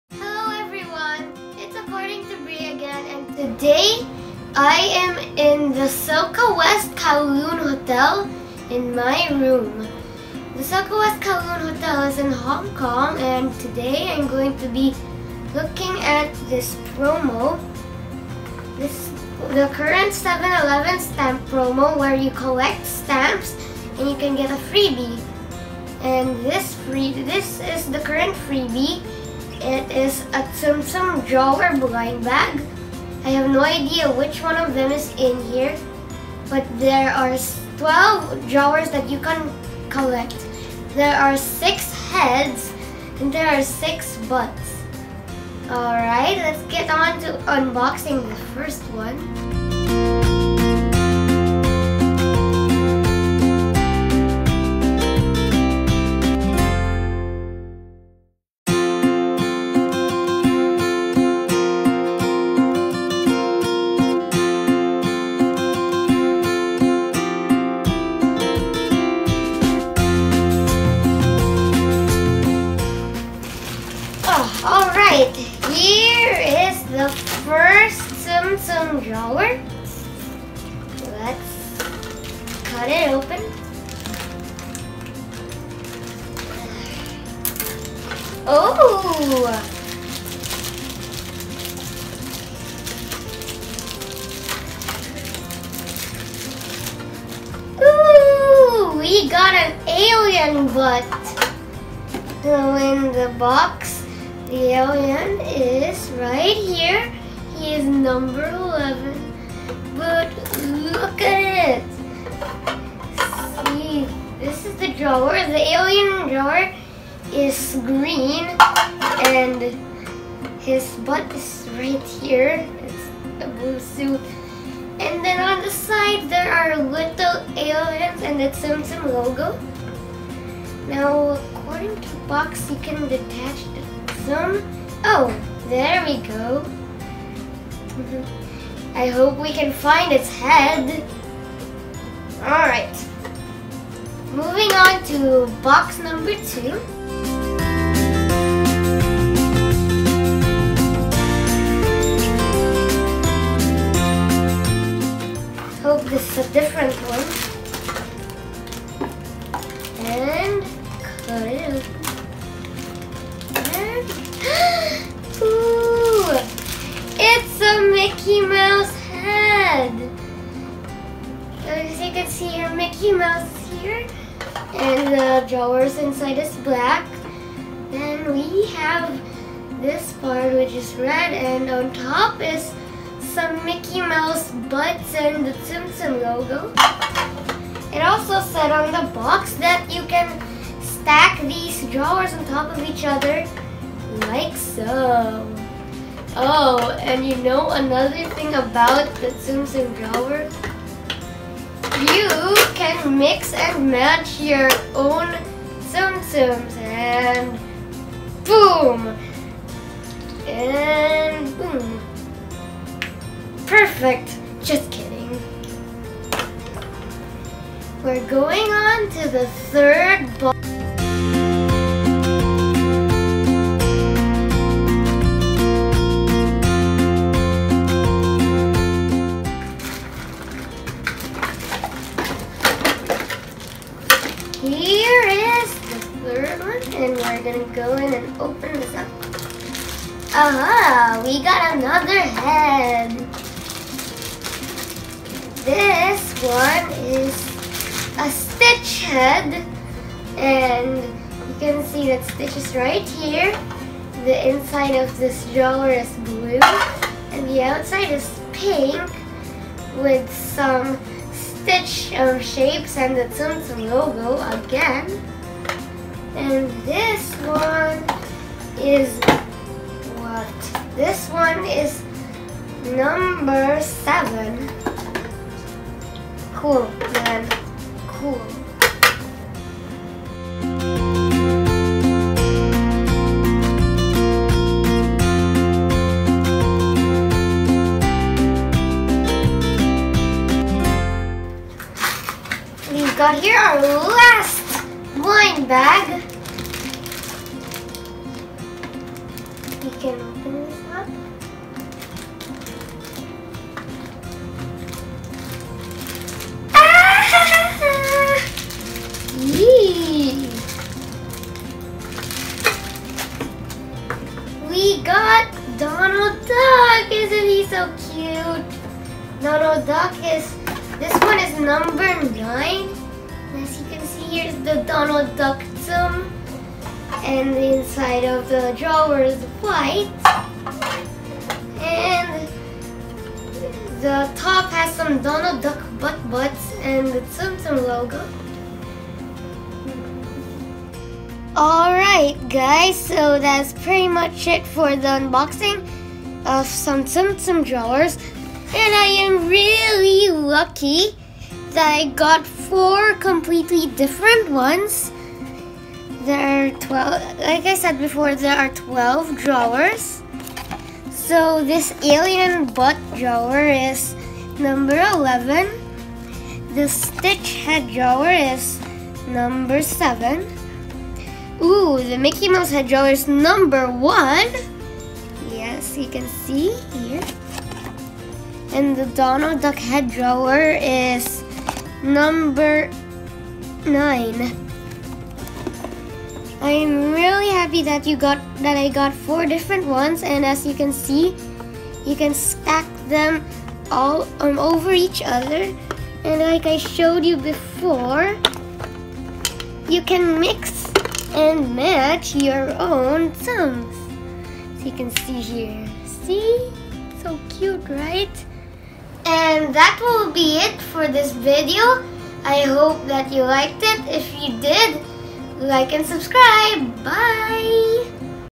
Unboxing Tsum Tsum Drawers
I unbox some Tsum Tsum drawers I got from 7-11.